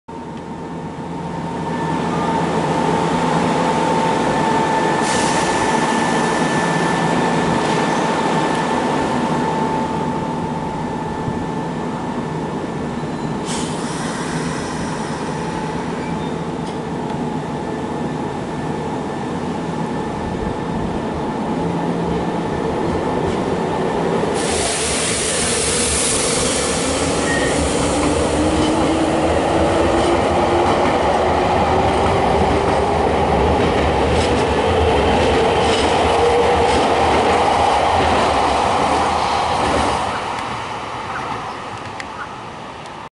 【泉北高速3000系】3523F+3521F 区間急行和泉中央ゆき 8両編成
天下茶屋駅発車シーン。
撮影日時：令和3年7月23日
撮影機材：HANDYCAM CX470
撮影場所：南海天下茶屋駅